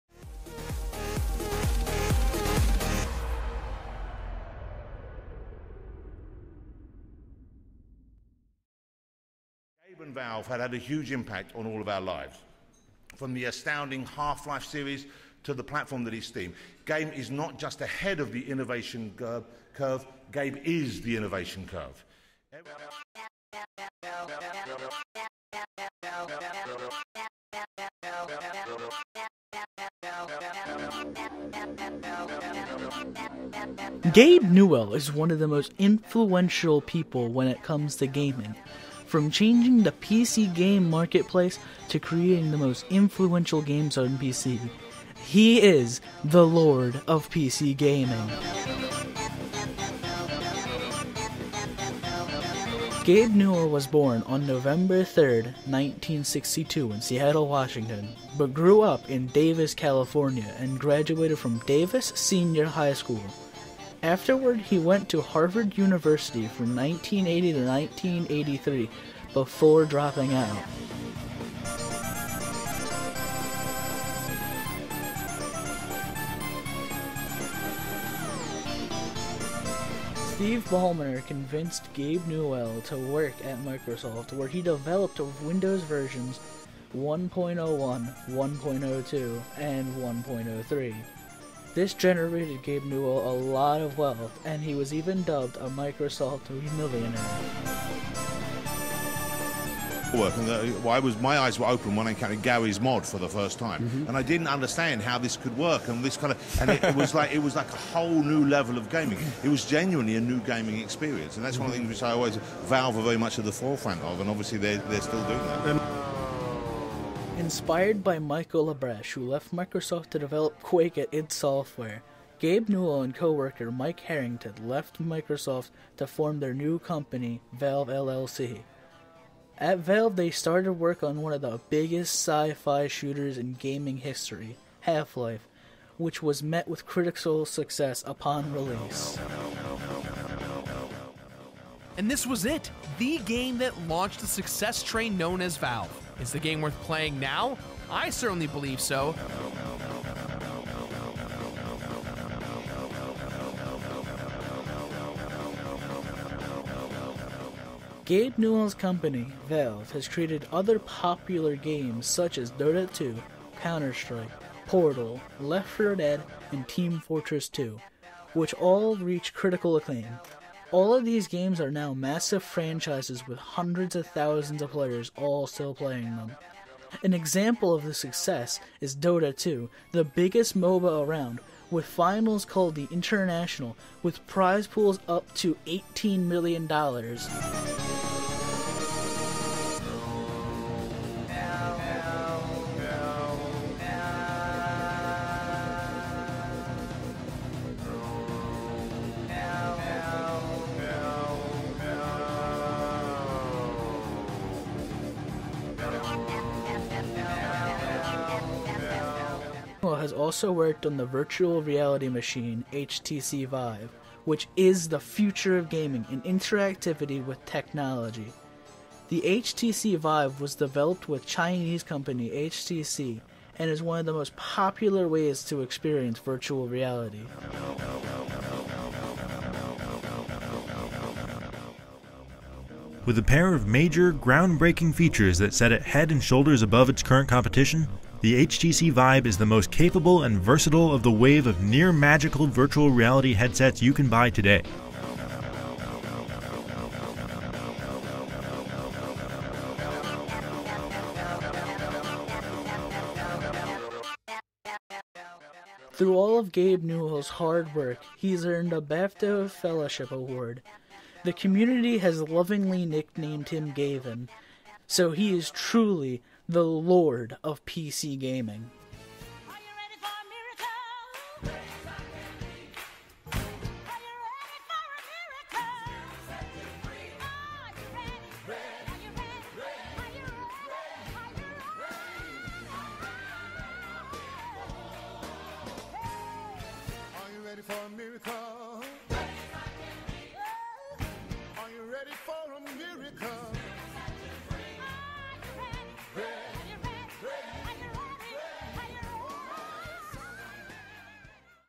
The Lord of PC Gaming | Gabe Newell Biography
Learn about the Lord of PC Gaming in this mini bio.